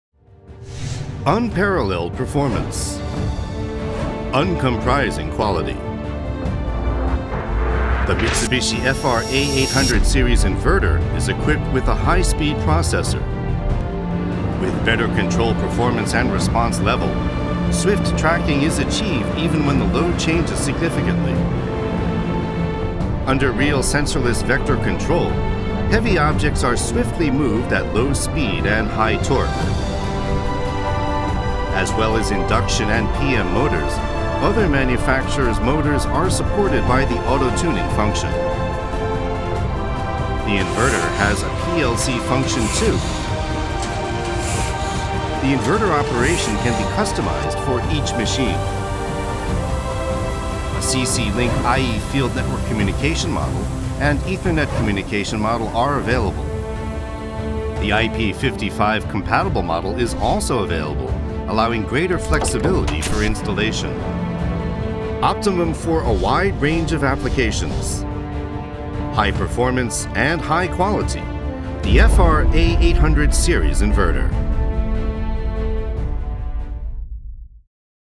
FR-A800
High performance and high quality new inverter of the highest level.
The FR-A series offer the best drive performance in the FR series.
The enhanced Real sensorless vector control and vector control serve the needs of all machinery types.